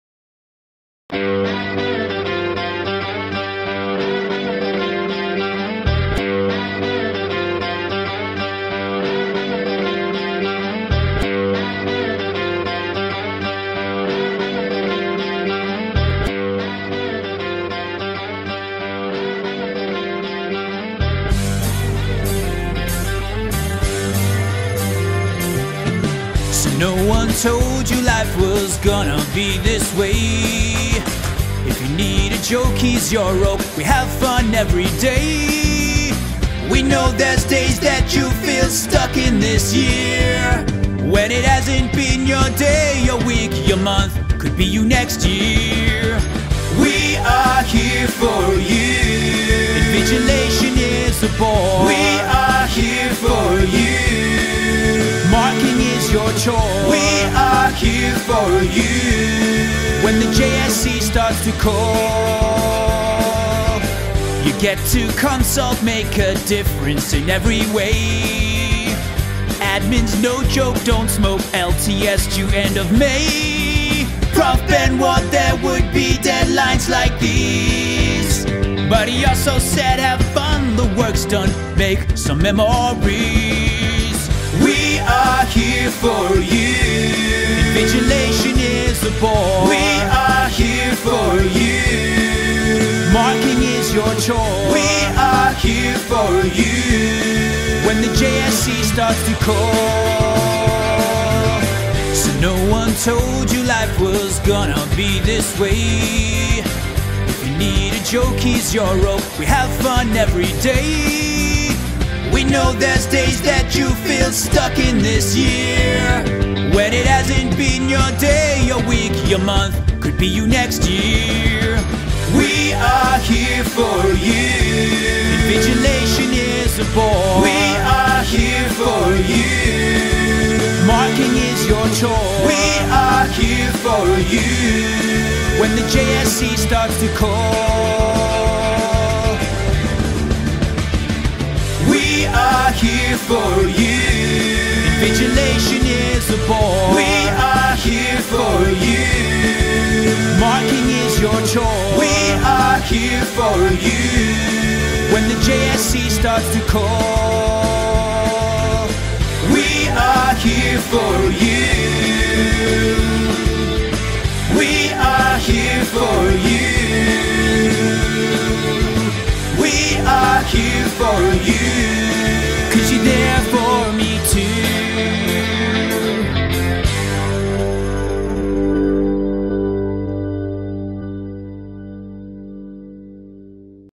Accountancy@UJ Academic Trainee Friends
Accountancy@UJ Academic Trainees 2019

Featuring our very own Mr Garth Barnes from CrashCarBurn on lead vocals...

Roseanne Chadinha
Zander Lategan
Pieter Van Zyl
Sahal Hoosen
Dani Friedman
Shane Bosch
Jesse Montano
Marco Lorenzo Pittich
Jazmyn Yerakiah
Morgan Coetzer
Gordon Sekome
Akshay Bhula
Ushmikka Badal
Lydia Molelekoa
Tshiamo Mothibe

A proudly Accountancy@UJ production